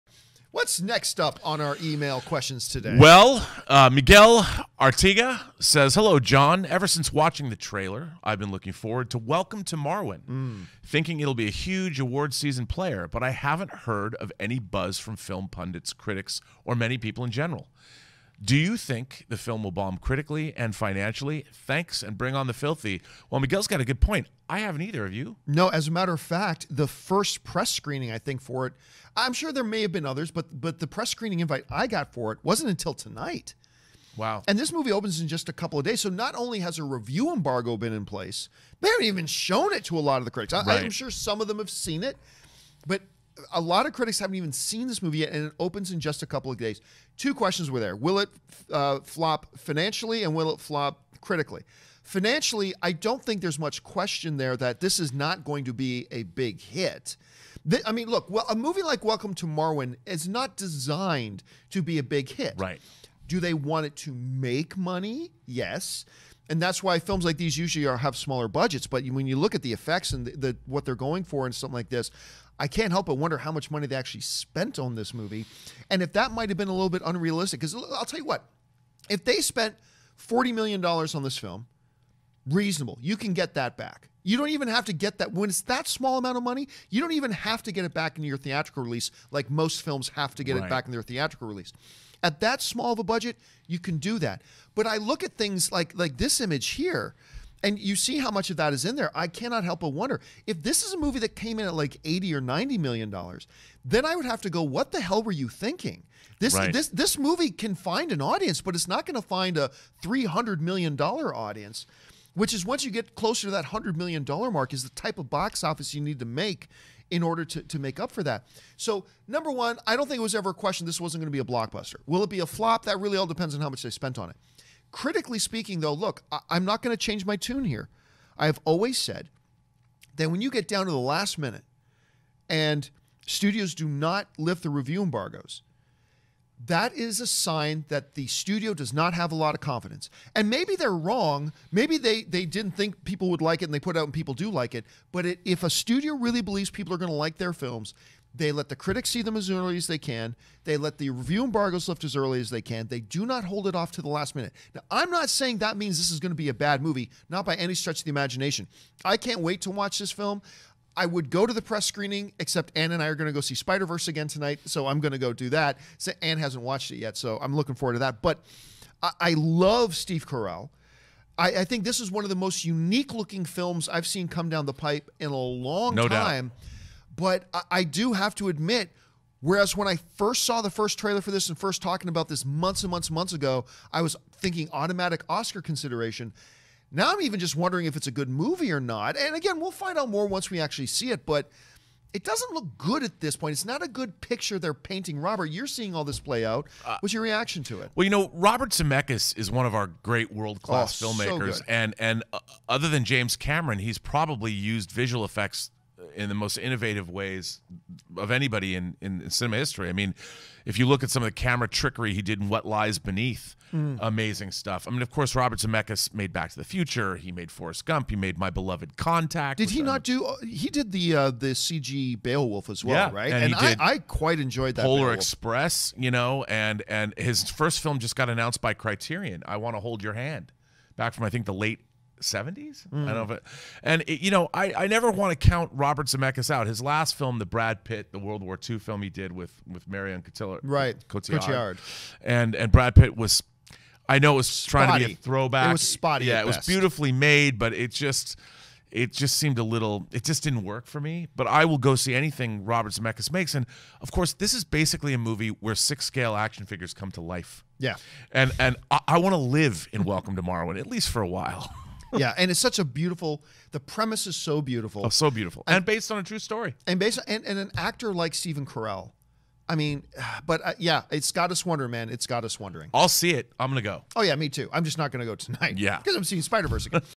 Is Welcome To Marwen Destined To Fail?
Miguel Arteaga - Hello John! Ever since watching the trailer, I've been looking forward to Welcome to Marwen, thinking it'll be a huge award season player. But I haven't heard of any buzz from film pundits/critics or many people in general. Do you think the film will bomb critically and financially? Thanks and bring on the filthy!.